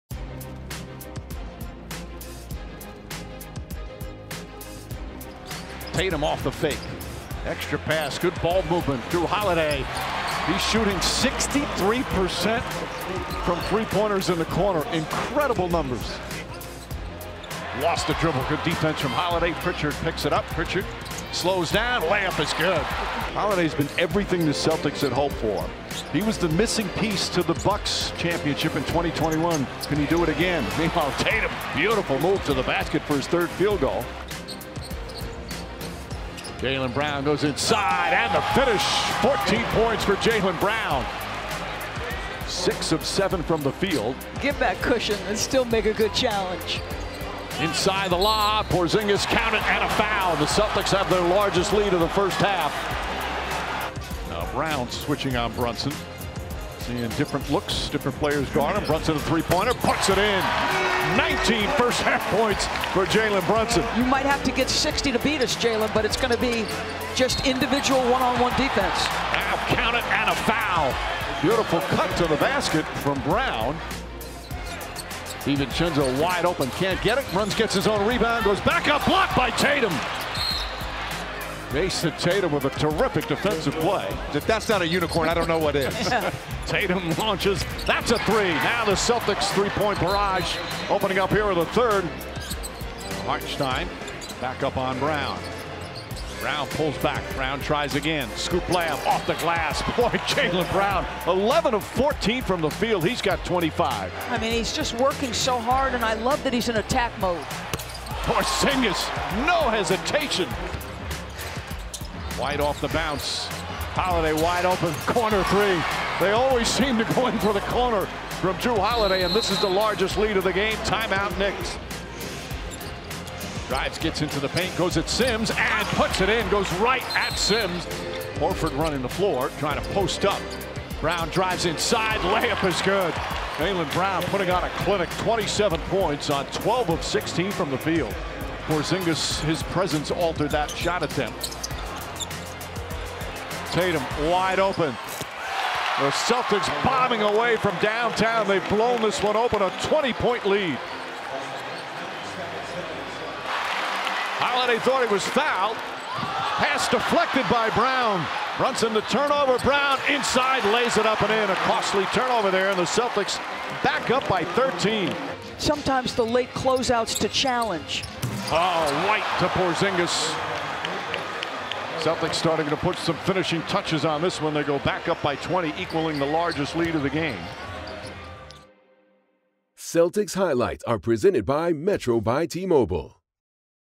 HIGHLIGHTS: Celtics shoot the lights out to knock off the Knicks for eighth straight win
Full game highlights from the Boston Celtics' 116-102 win over the New York Knicks. With the win, Boston improves to 45-12 on the season and increases its lead in the Easter Conference to 8 games.